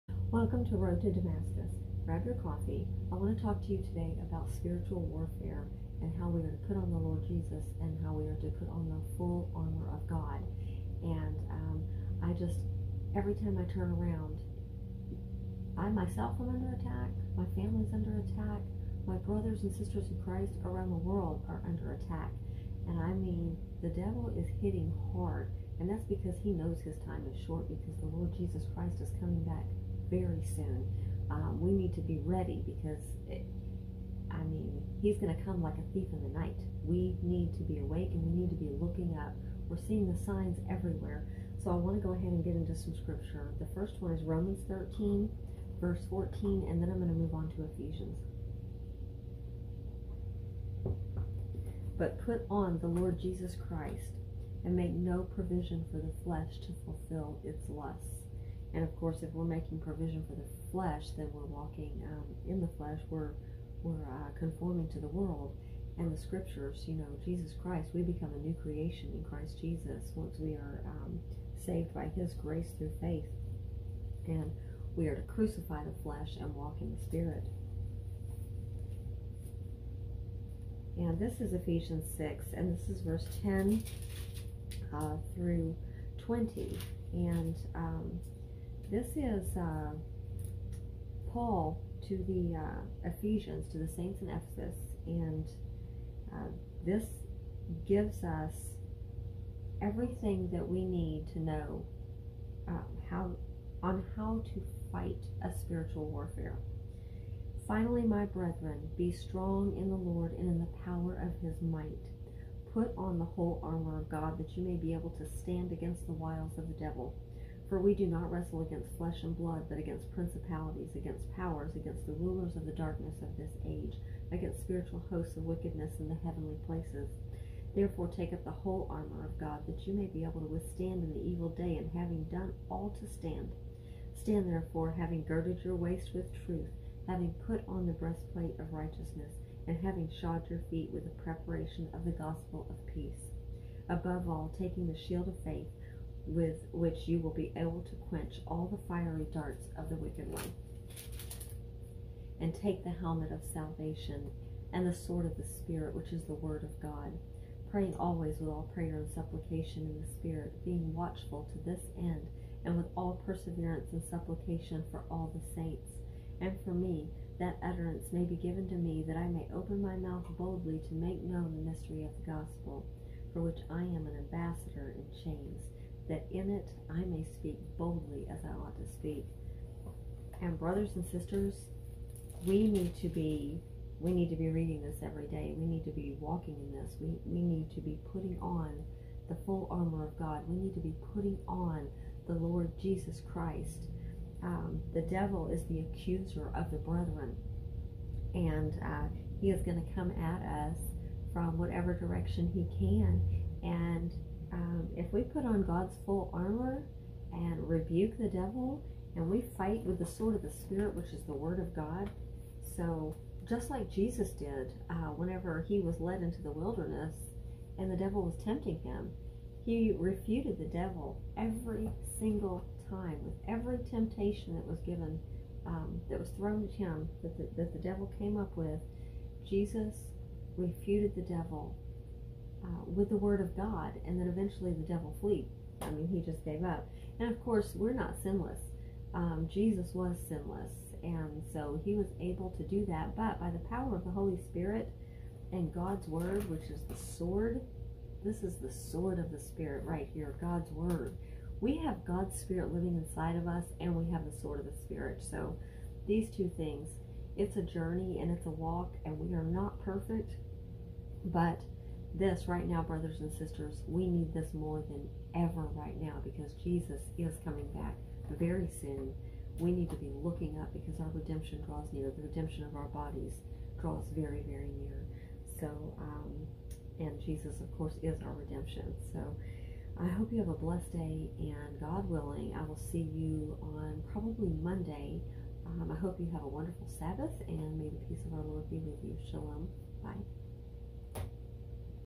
How to Fight Spiritual Warfare in These Last Days
Put on the Lord Jesus Christ and the Full armor of God.